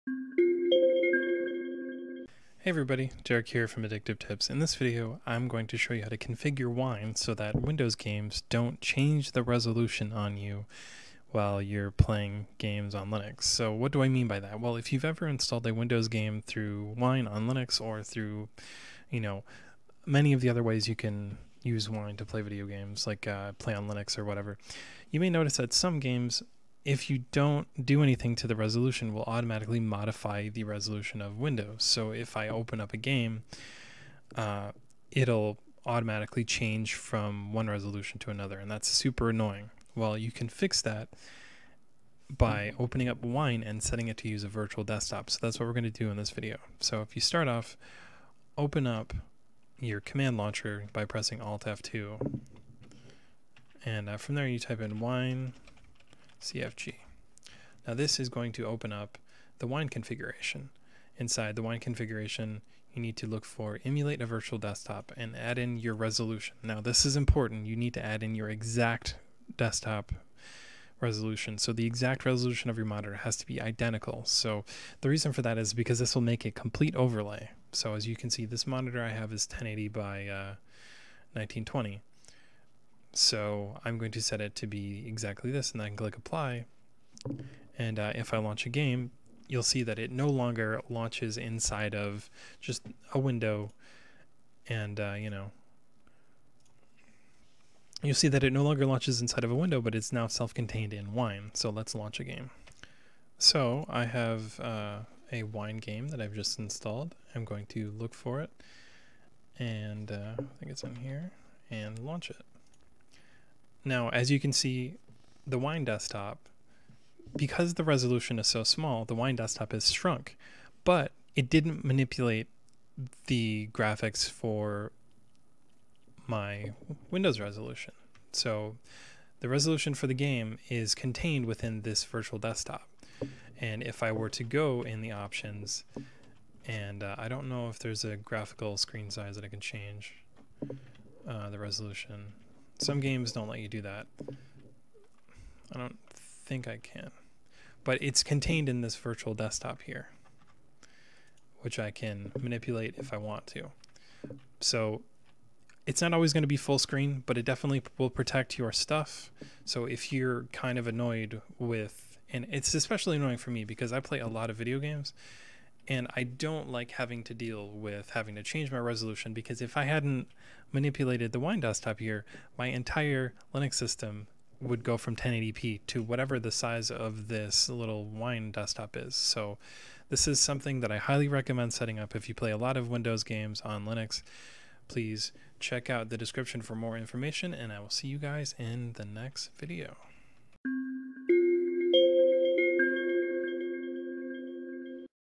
How to prevent Windows games from changing monitor resolution on Linux
A lot of Windows games, when running on Linux can manipulate the native display resolution of Linux desktop sessions and change them to the wrong size. Games switching resolutions is a severe annoyance, and it’s something that many Linux users would like to know how to fix. So, here’s how to prevent Windows games from changing monitor resolution on Linux.